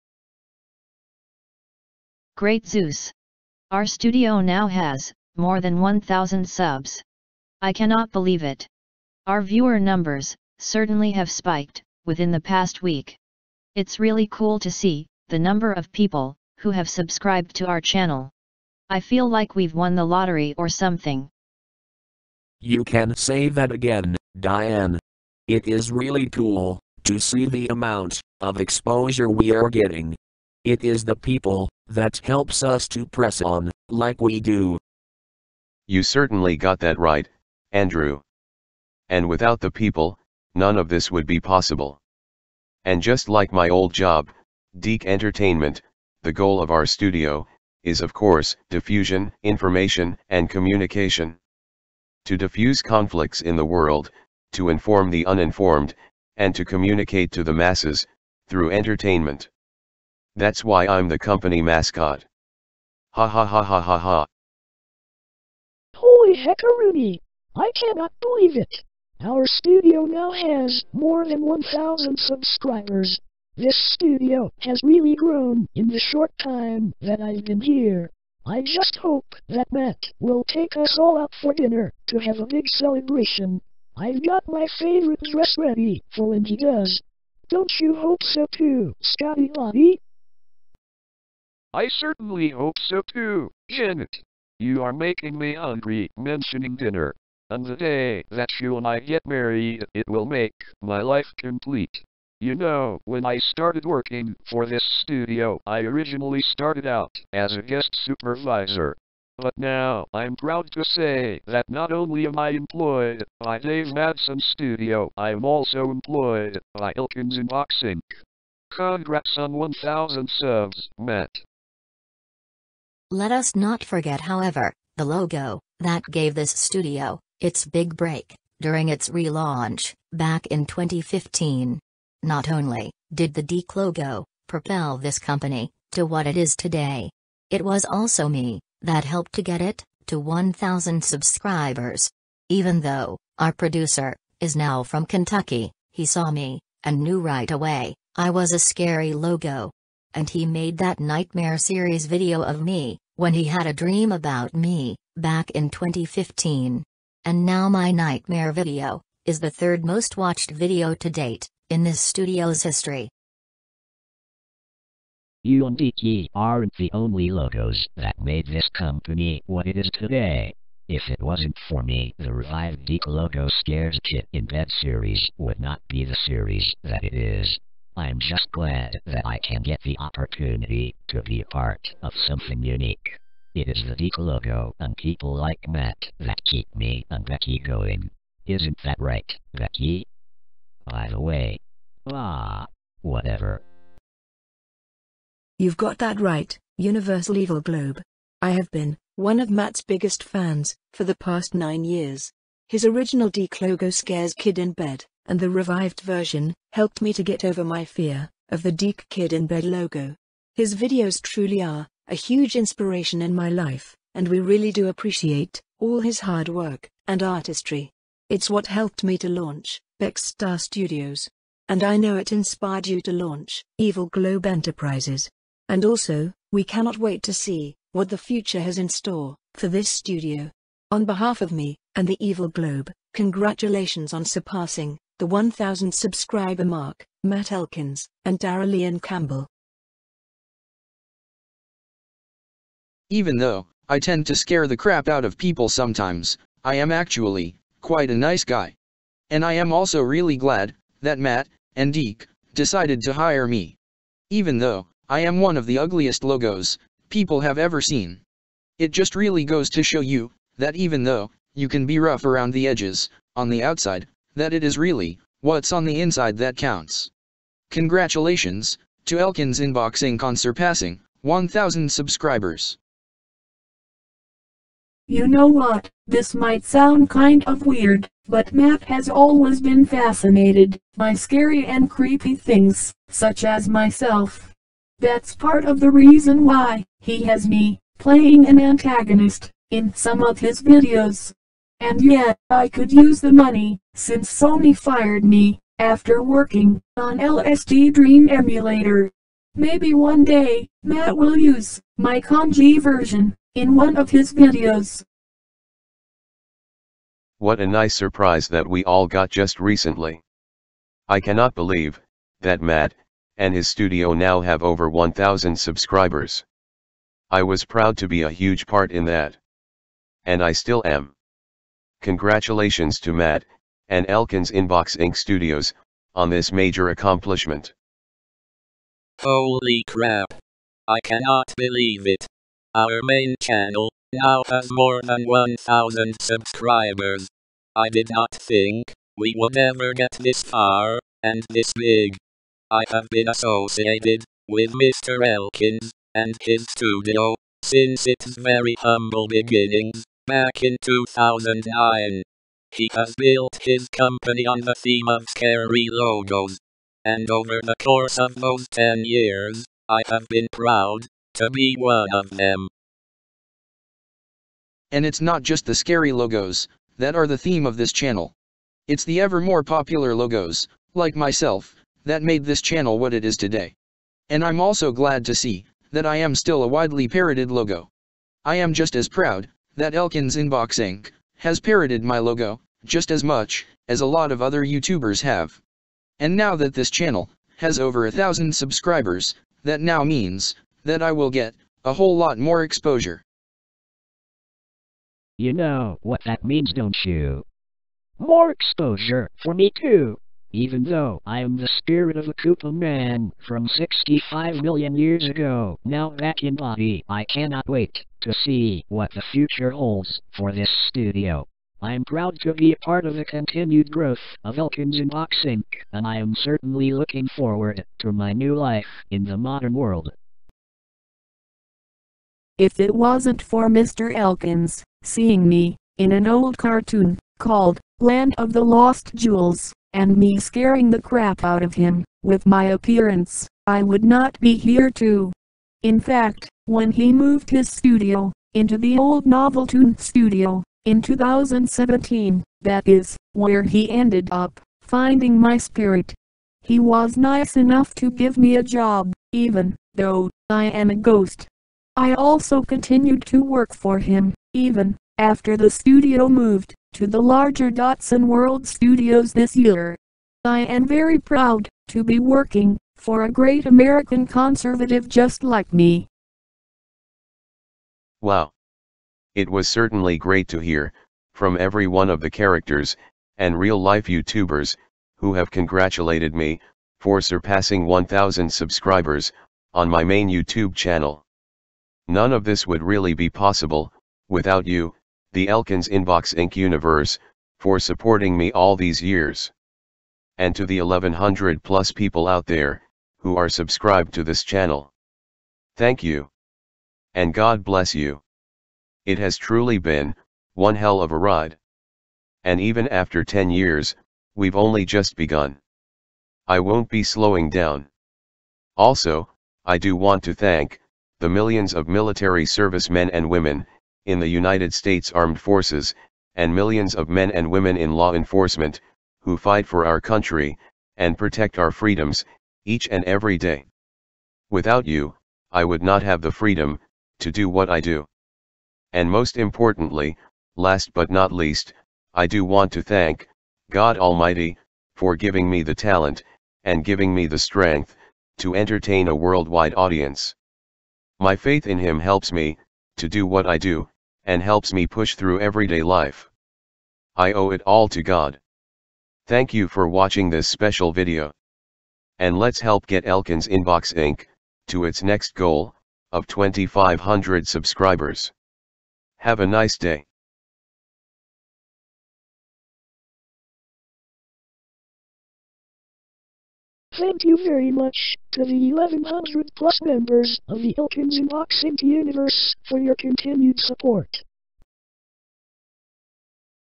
Thank You For Helping Me Surpass 1,000 Subscribers (110319A)
Thank you very much to the more than eleven hundred members of the Elkinsinboxinc Universe for helping me to surpass the 1,000 subscriber mark. I made this little video to celebrate this major accomplishment.

110319A